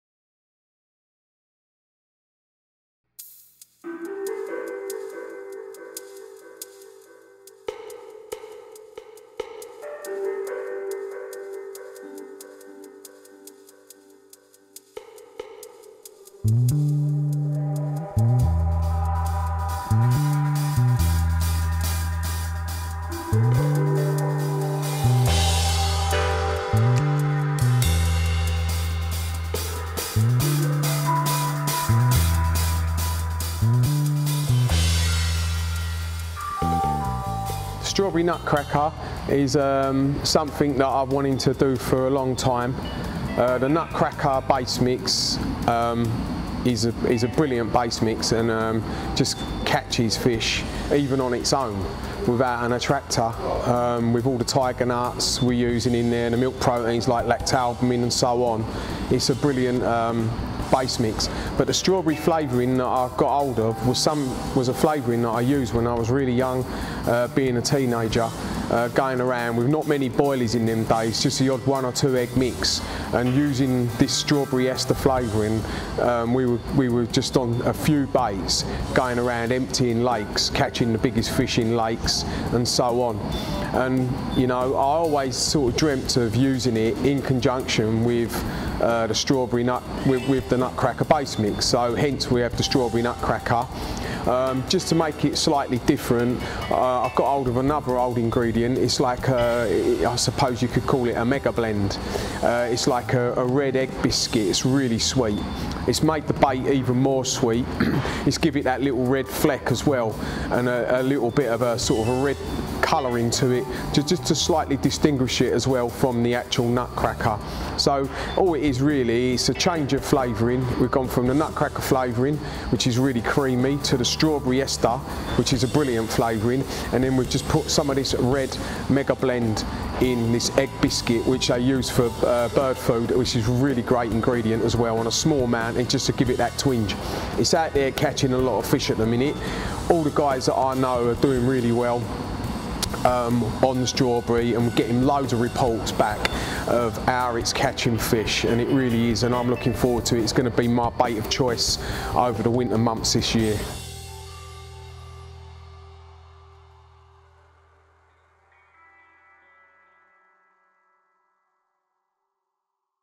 The Story of the Strawberry Nutcracker
Terry Dempsey talks you through the beginnings of the Strawberry Nutcracker and the great ingredients that go into this awesome bait! Make sure you get on the Strawberry Nutcracker this winter!